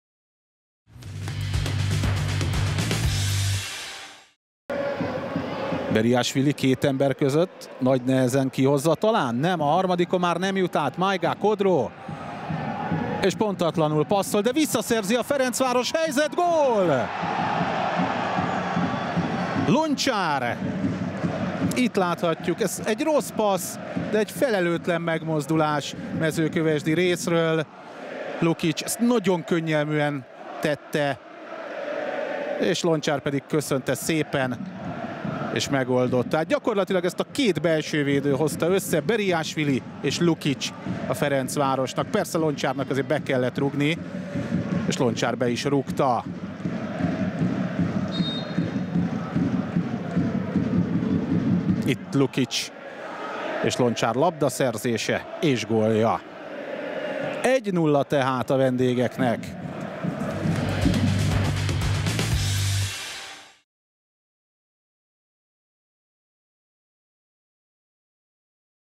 Loncar Stjepan első gólja a Mezőkövesd Zsóry FC - Ferencvárosi TC mérkőzésen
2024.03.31. OTP Bank Liga, 26. forduló
Mezőkövesd Zsóry FC - Ferencvárosi TC | 0-3 | (0-1) |
Loncar Stjepan első gólja